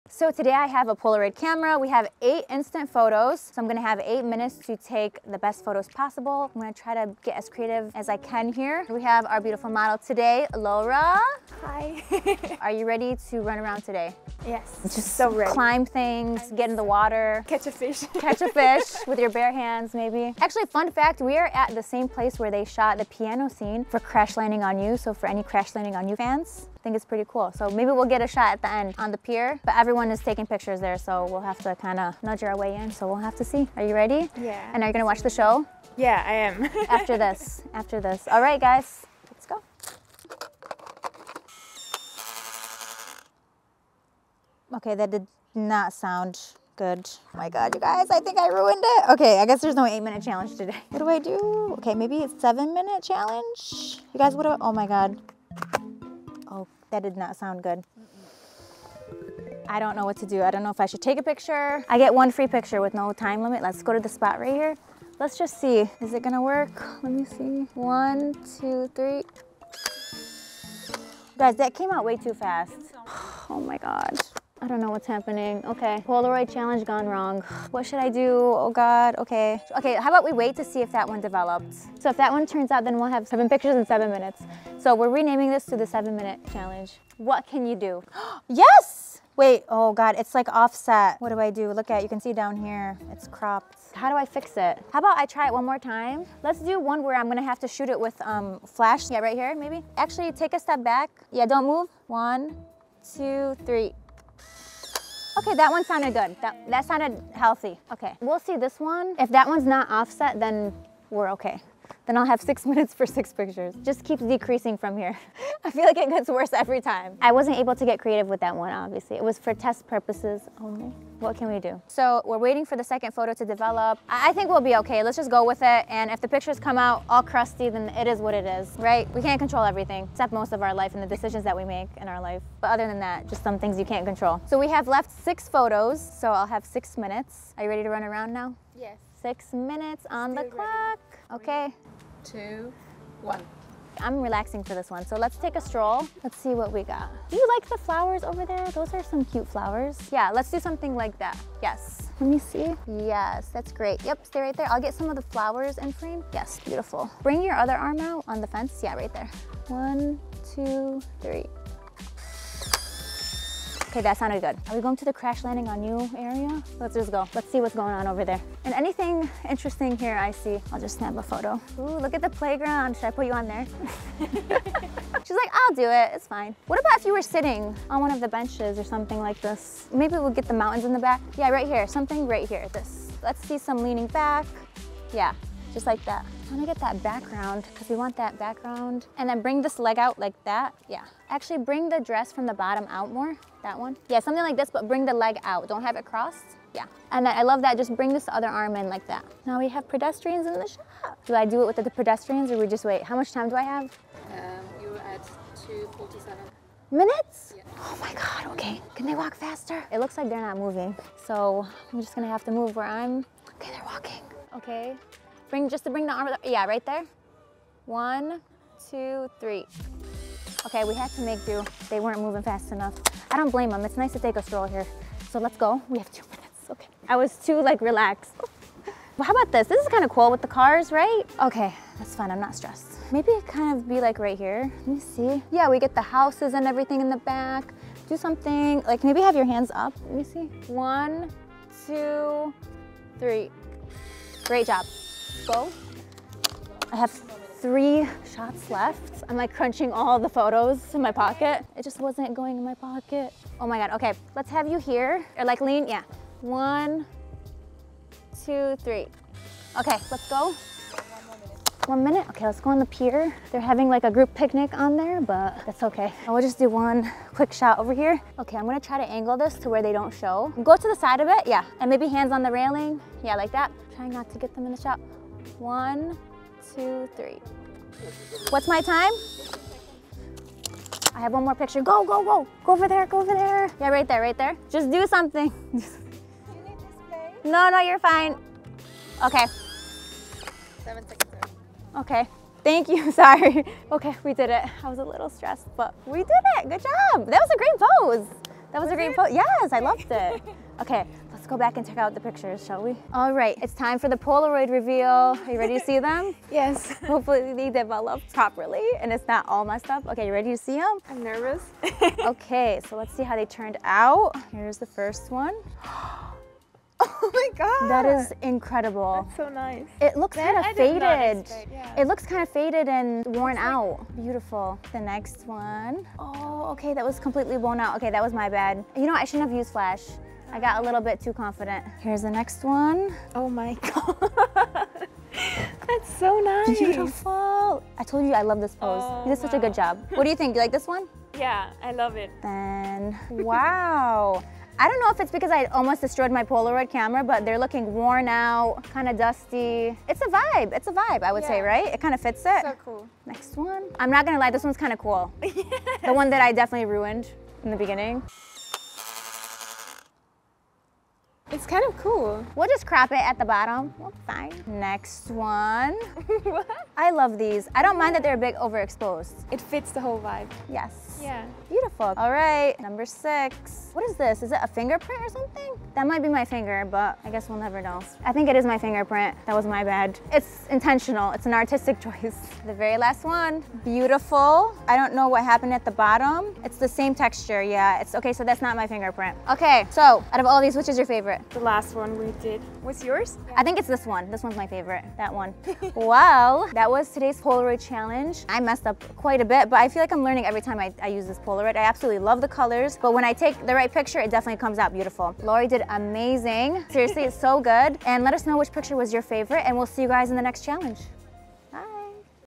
8 MINUTE POLAROID CHALLENGE GONE WRONG
We are at the crash landing for you special scene doing the 8 minute polaroid challenge...that turns into a 6 minute challenge. and so on lmao